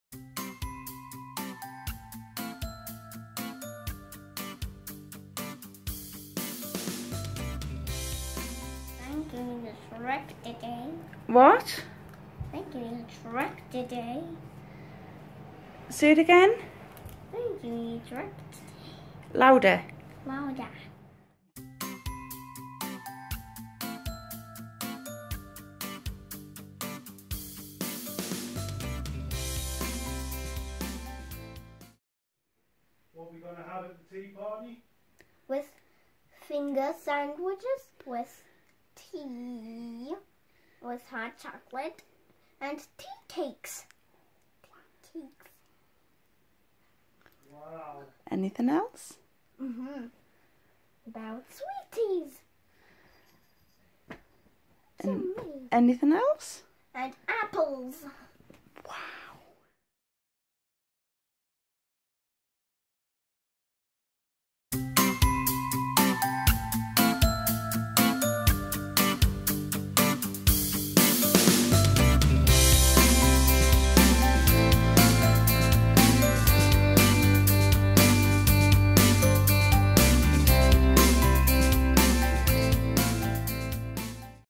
This six-year-old with autism was told she may never talk, read or write - just look at her now.
Thanks to pioneering teaching methods, Washington youngster, Brooke Nacca is is now gearing up for her SATs.

Mum, Faye Nacca said: “It’s like a miracle for us. There was a time when we worried when we thought about the future. Would she be able to read and write, could she live alone? Everything seemed impossible until she was given a place at the North East Autism Society school in Sunderland.”